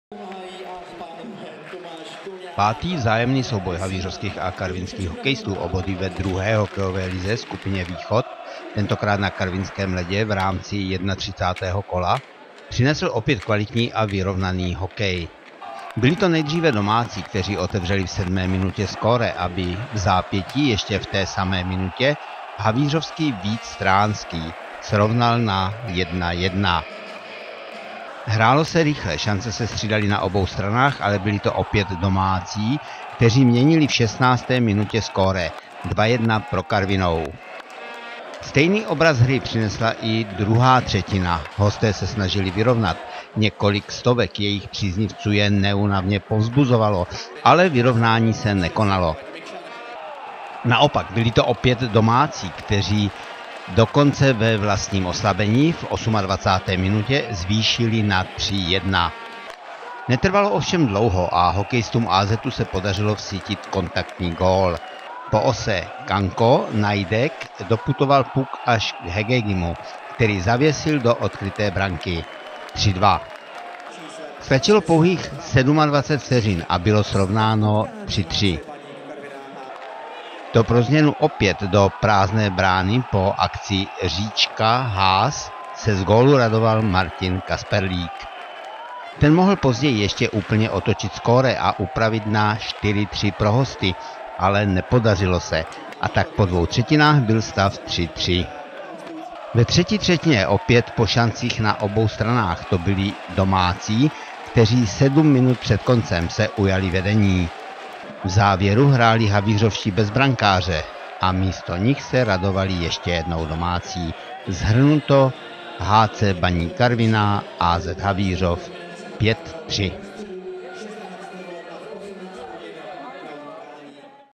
HC Baník Karviná - AZ Havířov 5:3 - 9. ledna
Vzhledem k hracímu systému druhé hokejové ligy, skupiny východ, svedl los dohromady k třetímu vzájemnému utkání v průběhu necelých čtrnácti dnů hokejisty sousedních měst, Havířova a Karviné, tentokrát na karvinském ledě.